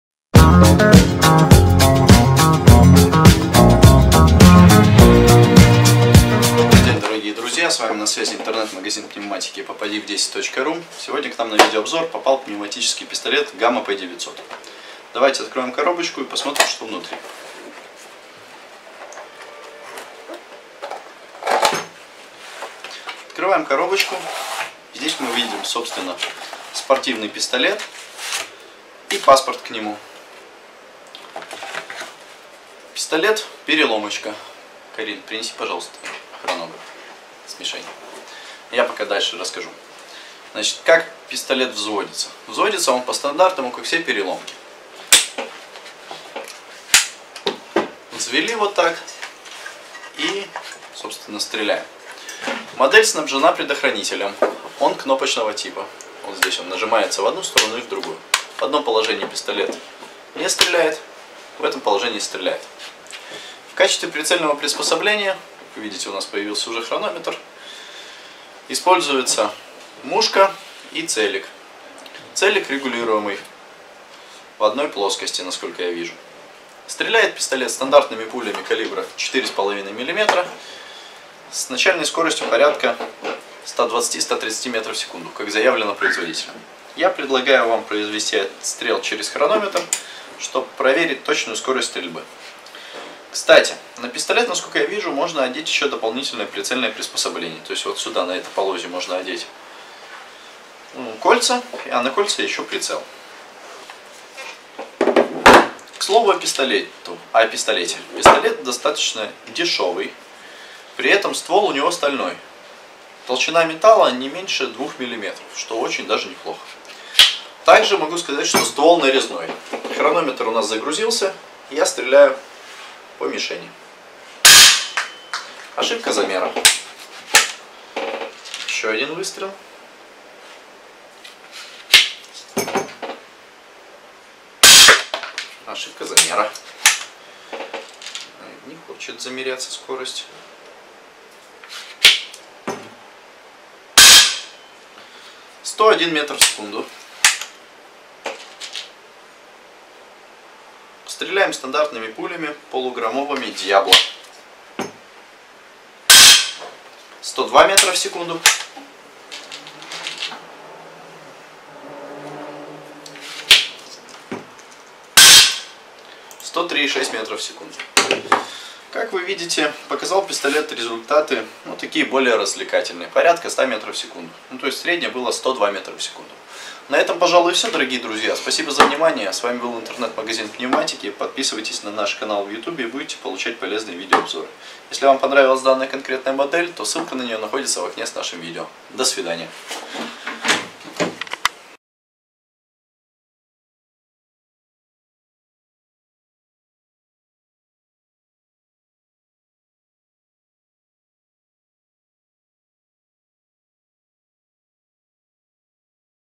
Пневматический пистолет Gamo p900 купить popadiv10.ru
Видео обзор на Пневматический пистолет Gamo p900, купить который можно в нашем интернет магазине popadiv10.ru